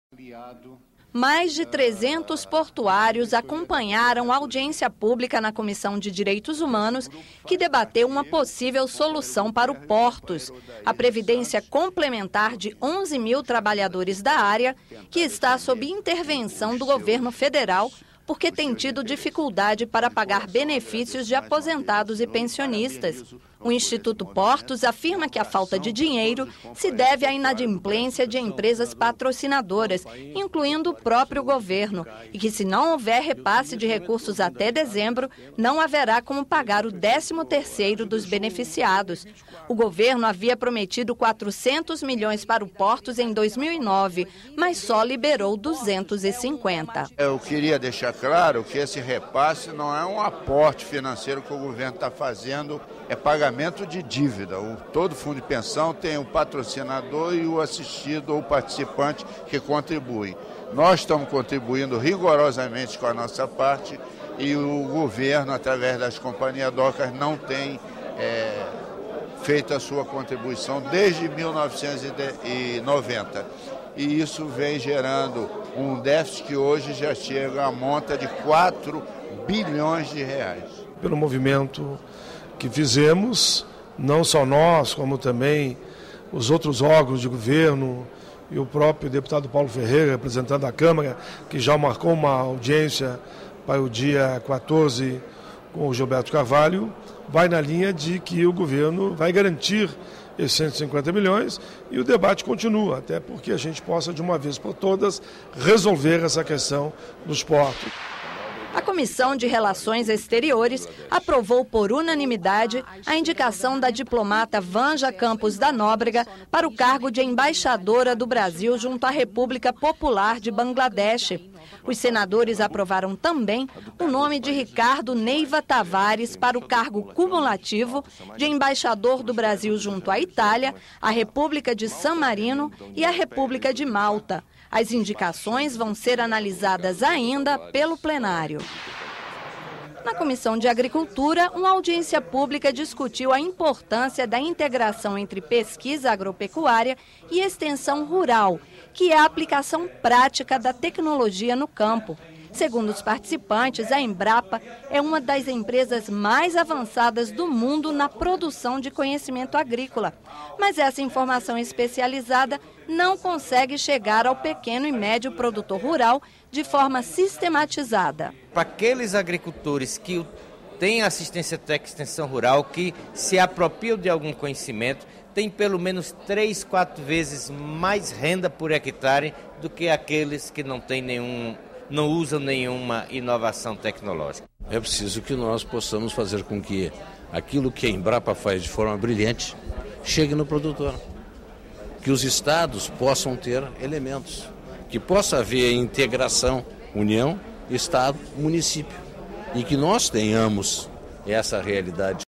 Jornalismo - Veja outros assuntos discutidos em comissões do Senado - Bloco 1
A aplicação de conhecimento científico na agricultura familiar; a indicação de novos embaixadores; e a situação do Fundo de Previdência dos Portuários